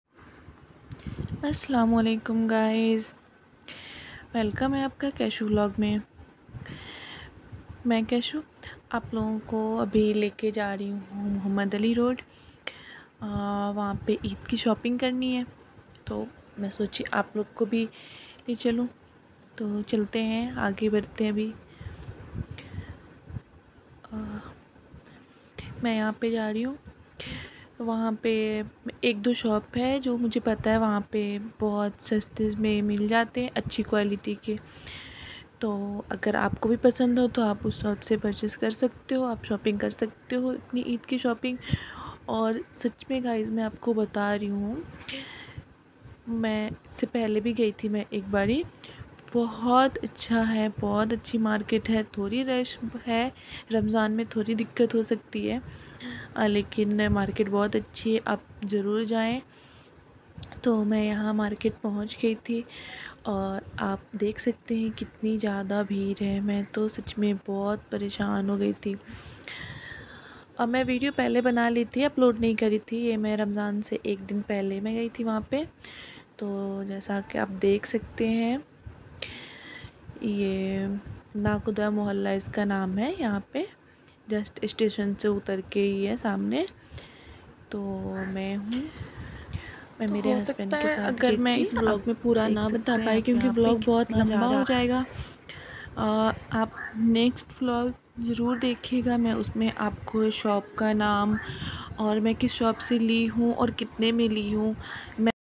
Shopping and Vlogging #01 Mohammad Ali Road ki shopping
Mohammad Ali Road pe ki Eid ki shopping.